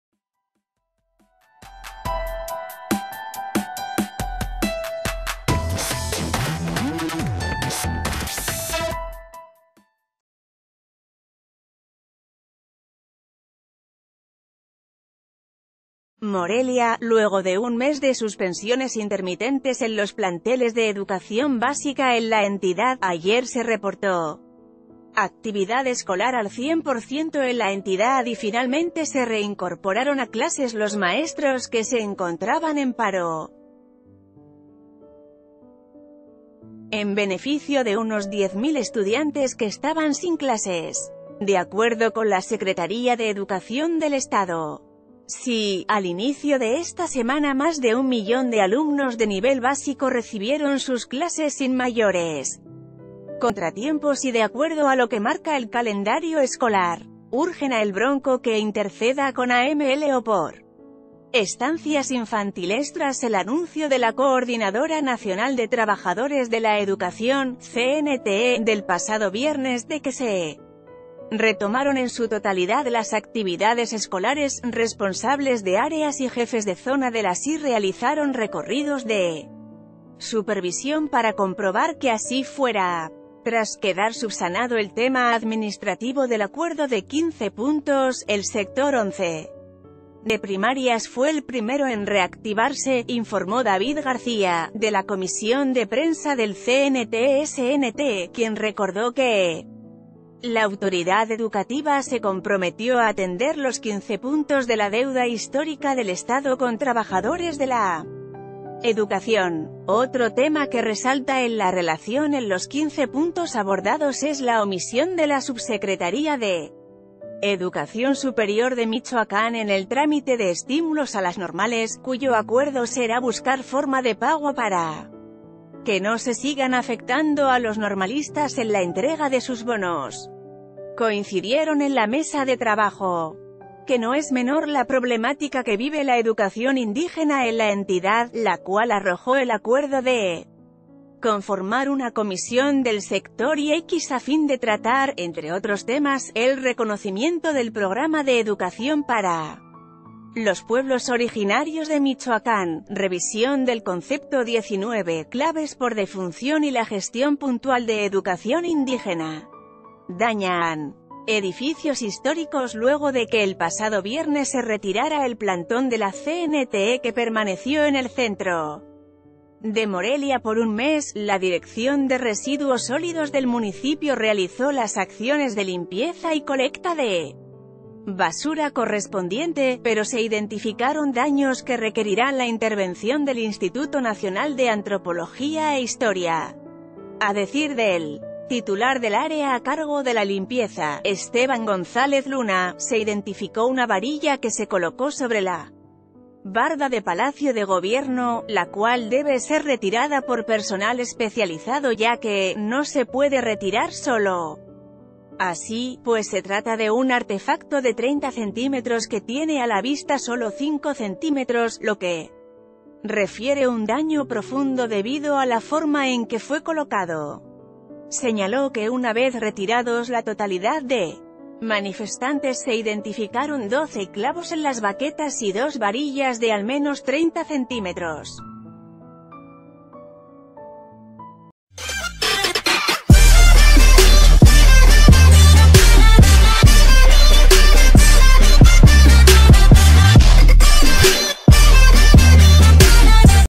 Regresan a clases 10 mil estudiantes en Michoacán
MORELIA.- Luego de un mes de suspensiones intermitentes en los planteles de educación básica en la entidad, ayer se reportó actividad escolar al 100% en la entidad y finalmente se reincorporaron a clases los maestros que se encontraban en paro, en beneficio de unos 10 mil estudiantes que estaban sin...